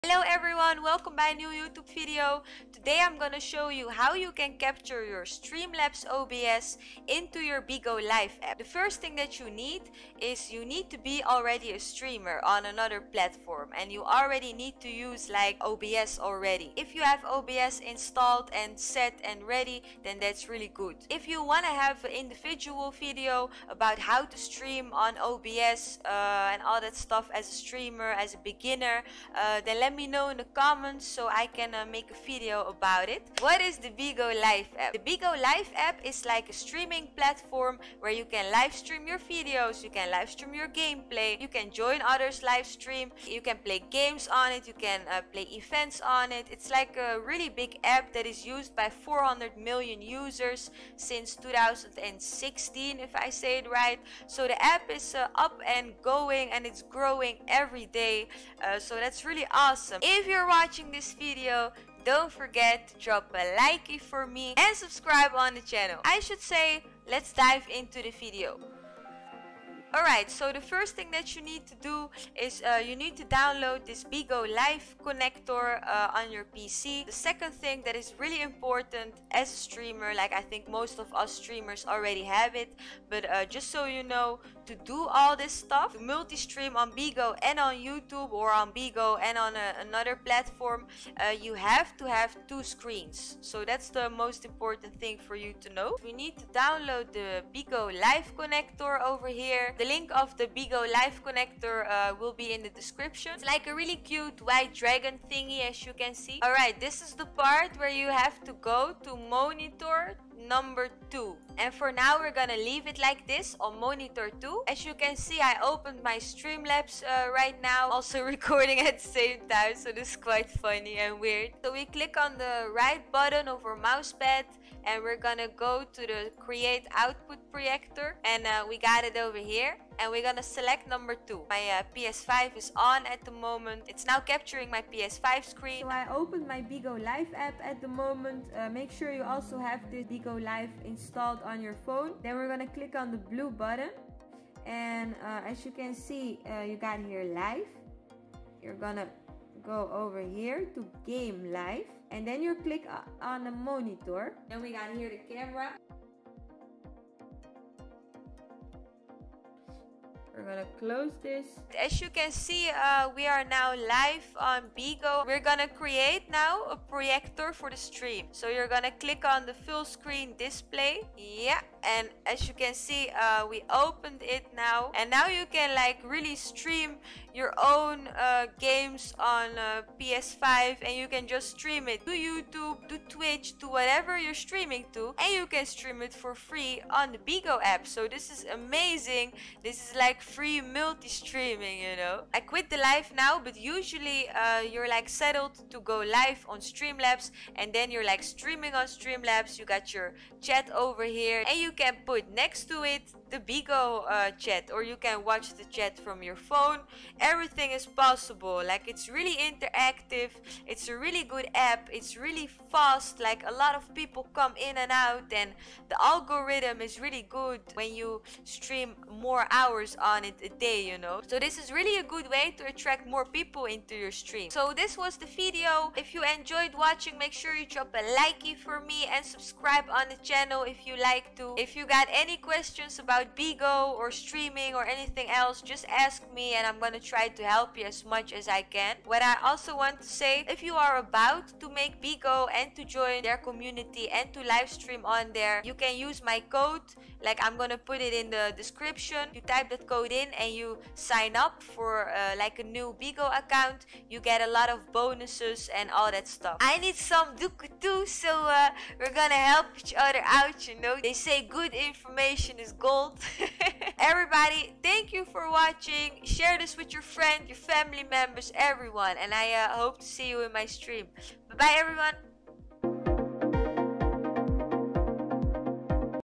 CAPTURE YOUR STREAMLABS OBS INTO YOUR BIGO LIVE APP! MULTISTREAM FREE PS5/XBOX/PC
The best way to grow is to stream on multiple platforms and try out what works best for you and your audience.